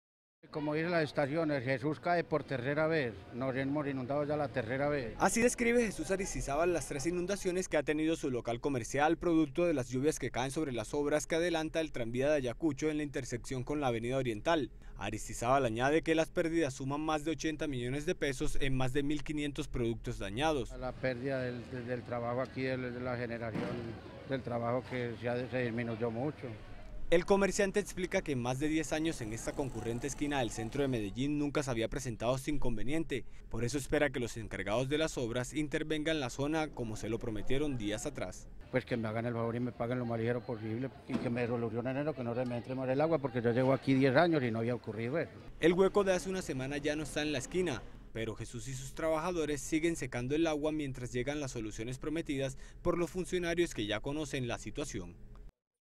Nuevas denuncias de inundaciones por obras del Tranvía en Ayacucho [Noticias] - TeleMedellin
El Lunes Noticias Telemedellín visitó un local comercial en el centro de Medellín que ha sido afectado por las obras del tranvía de Ayacucho. Los funcionarios de la empresa contratistas prometieron soluciones, pero dos días después, el local sigue inundándose.